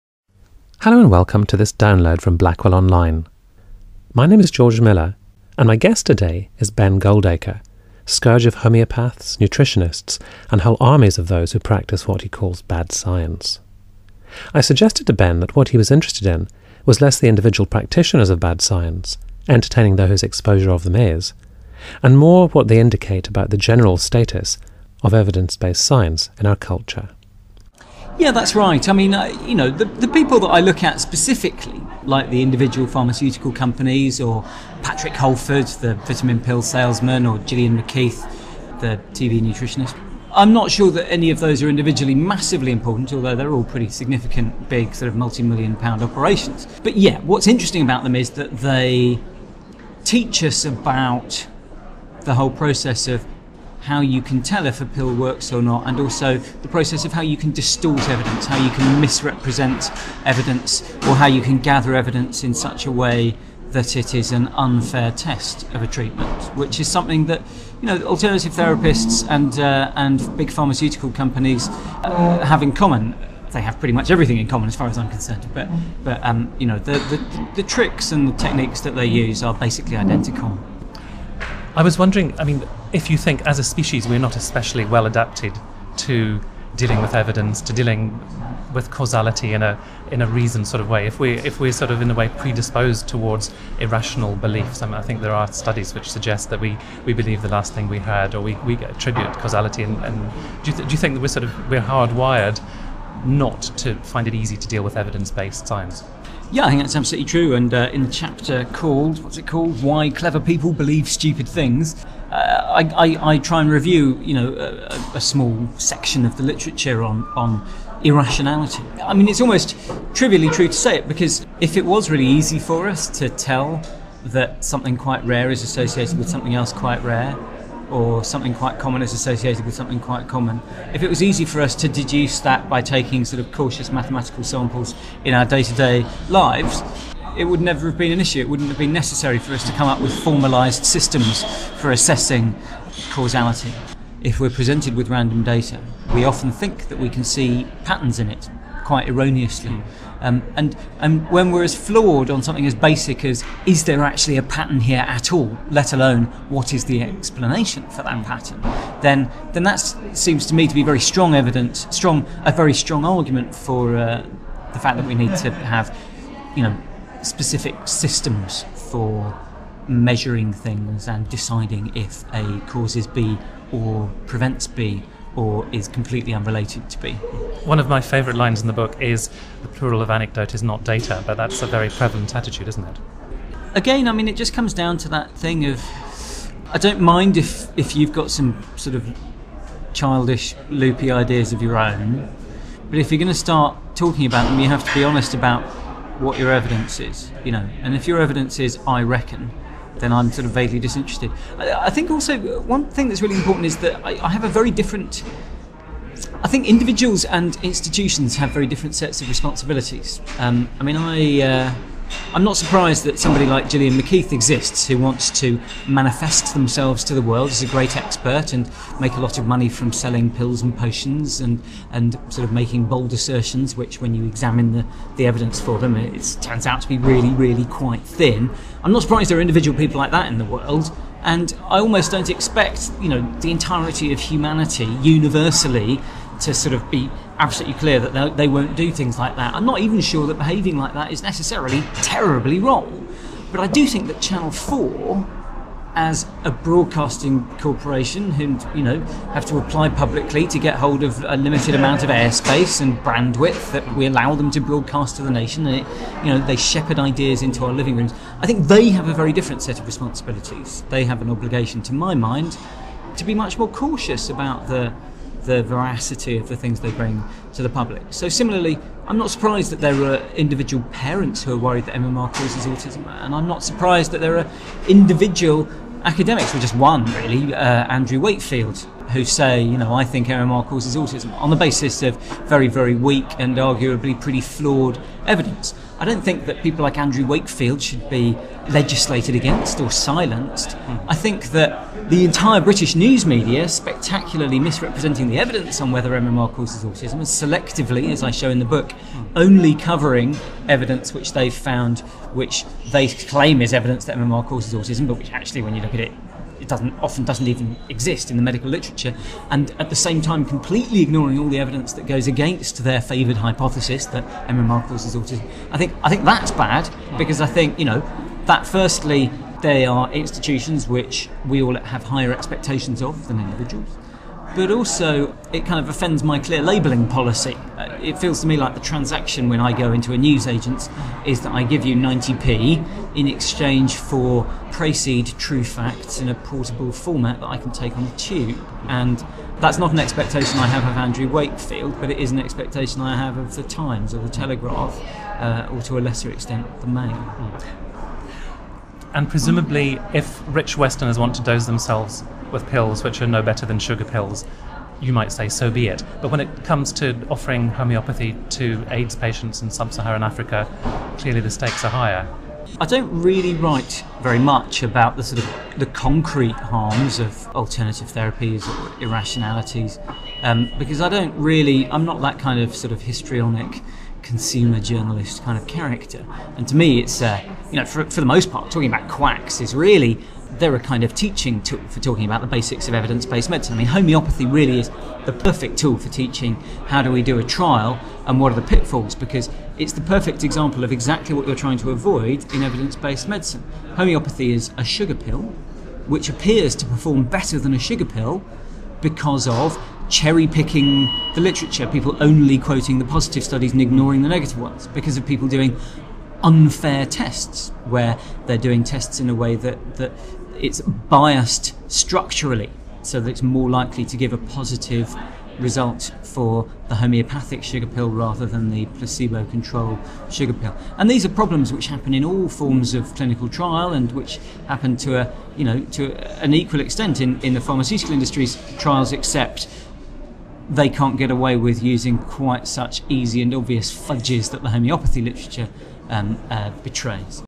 Ben Goldacre - Bad Science - Part 1 of 2
Ben Goldacre discusses his bestseller in which he debunks (with consistent hilarity) some of the most outrageous 'scientific' claims that we are bombarded with through the media today.